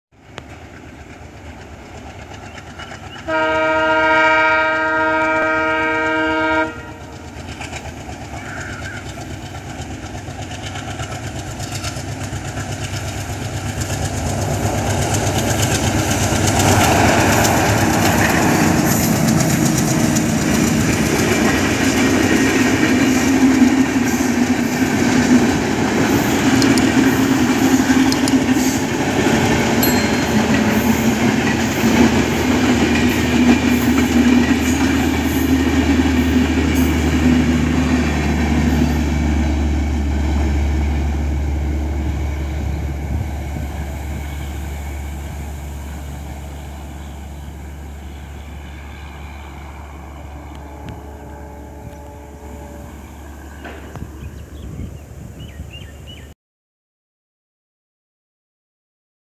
THE TRAIN RUNS TO IT'S DESTINATION IN A BEAUTIFUL CLEAN ENVIRONMENT || BR
The train runs to its destination in a beautiful clean environment of Bangladesh.This is very imagine that will be fine.